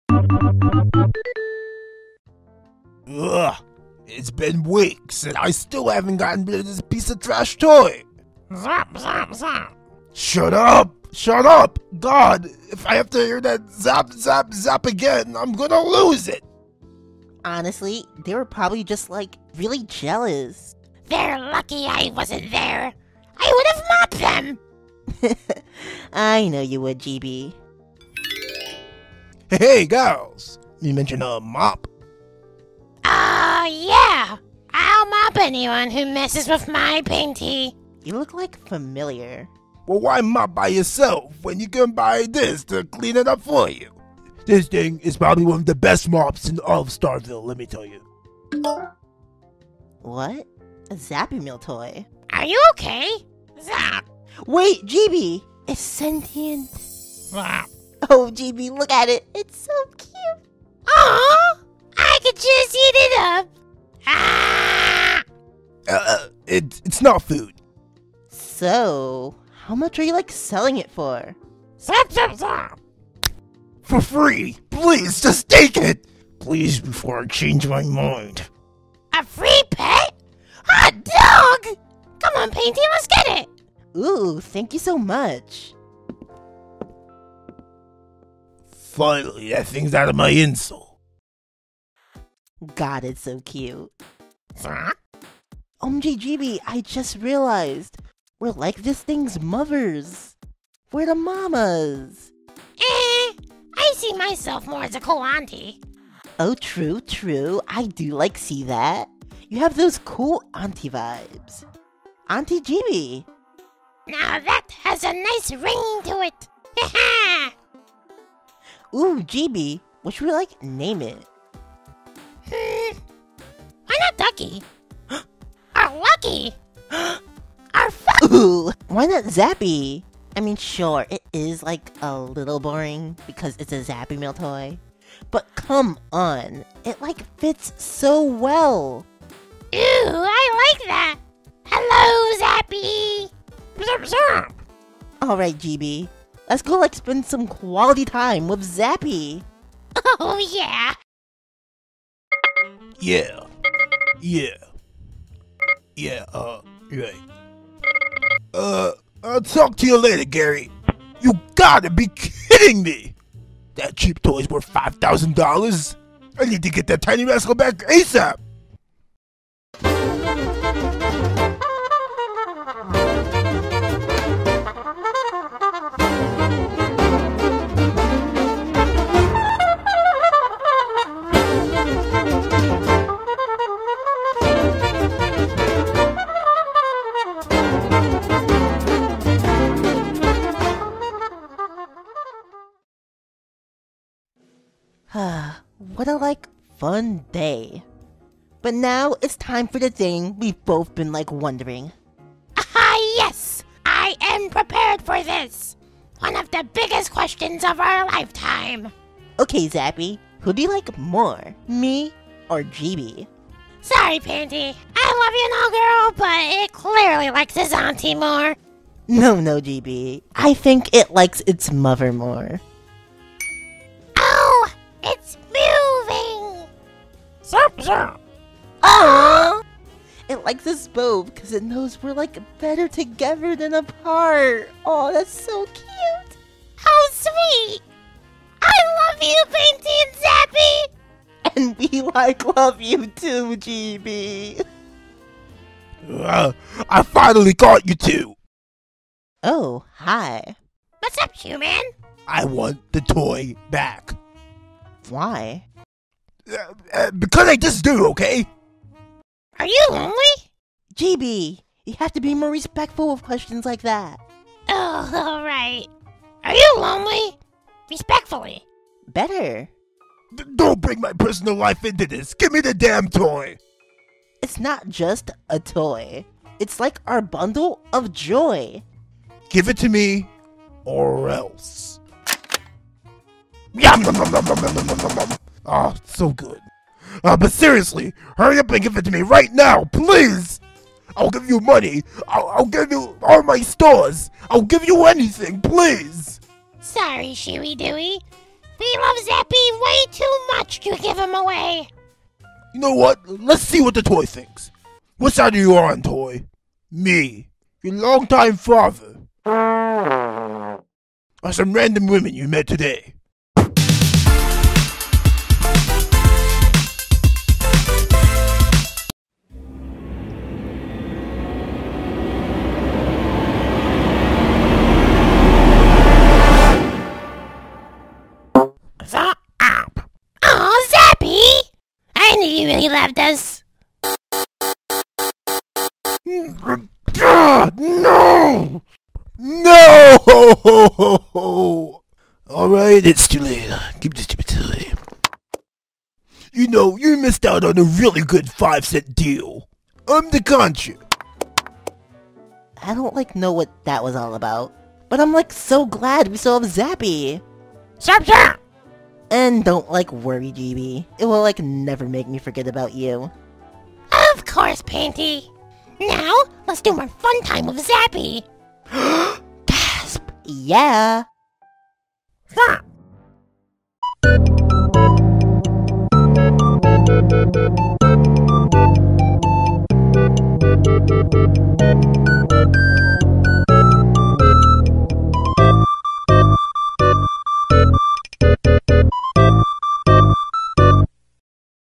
PB & GB Episode 6: "A Very Zappy Family"
In this episode of PB & GB, the girls find a seller of some mop...or is it a toy...a pet? A pet that's a toy that can mop maybe? Who knows, well I do, but watch!